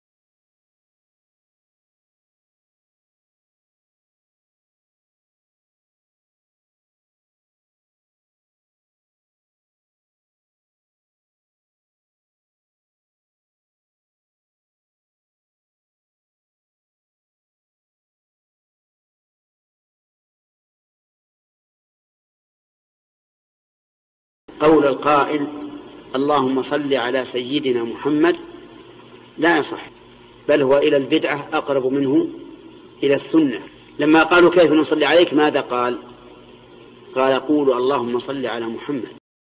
Celui qui dit : «اللهم صلِّ على سيدنا محمد - Allahoumma salli 'alã sayydinâ Mohammad»
Cheikh Mouhammad Ibn Salih Al-’Outheymine - رحمه الله